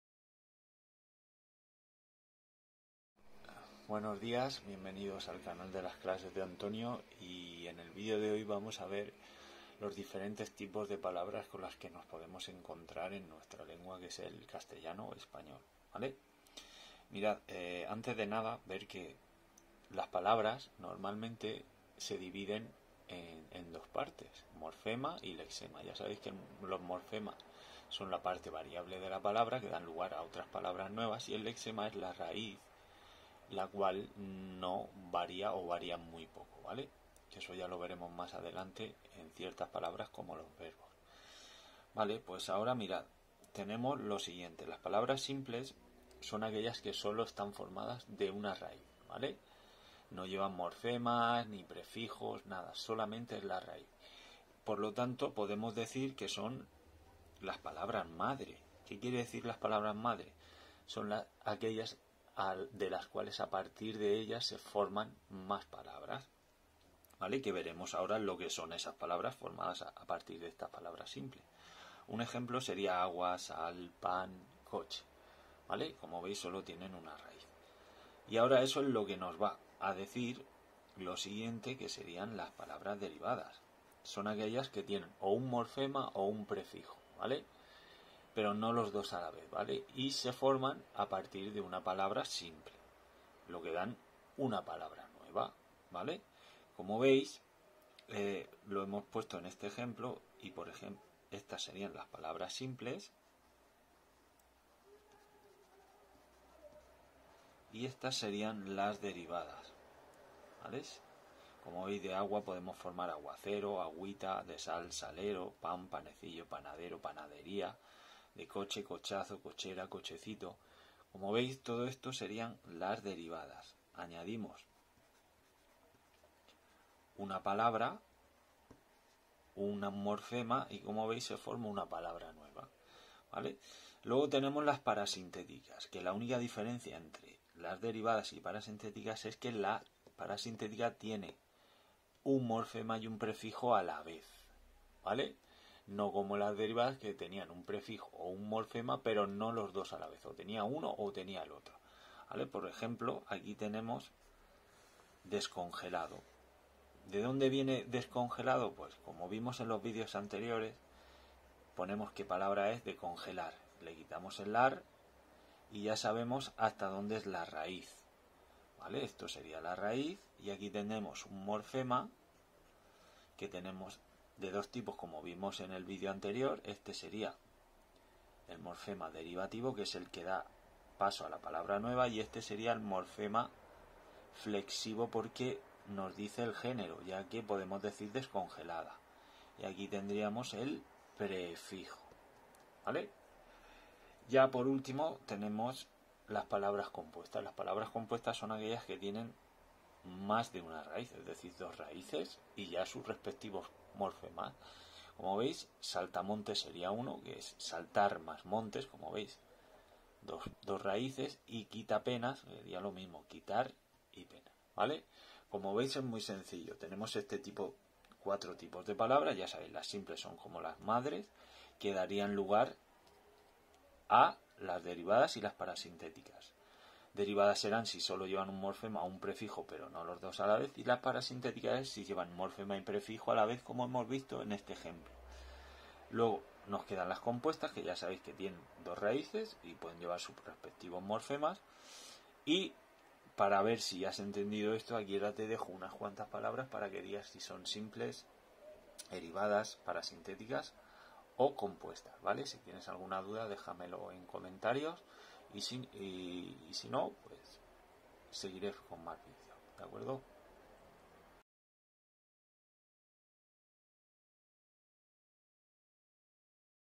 Palabras simples, derivadas, parasintéticas y compuestas
Tipos de palabras según las características de su raíz, morfemas y prefijos